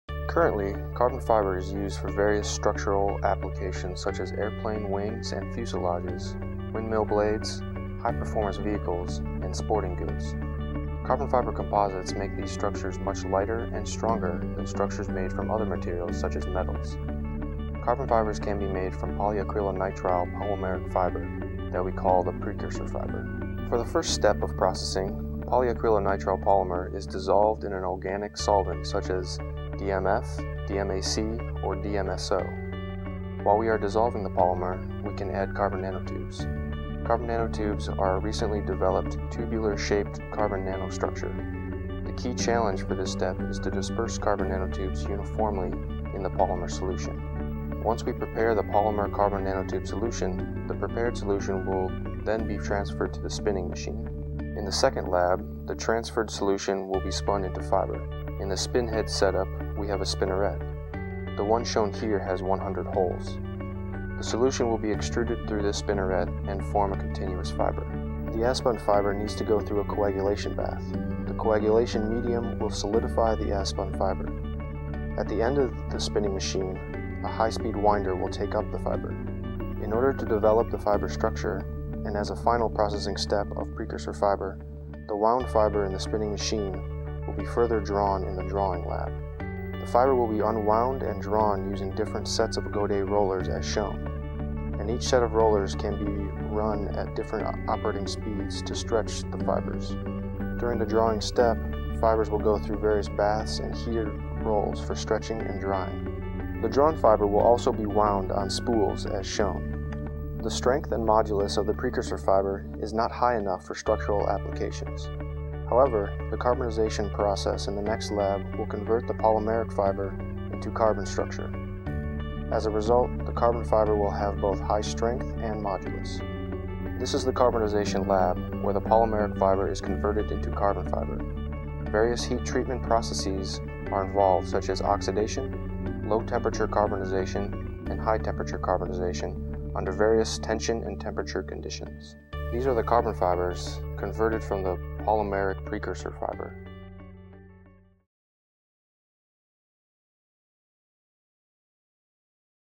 Carbon Fiber Lab at Georgia Tech
Learn how carbon fiber is produced in a DARPA-funded lab here on  Georgia Tech's campus.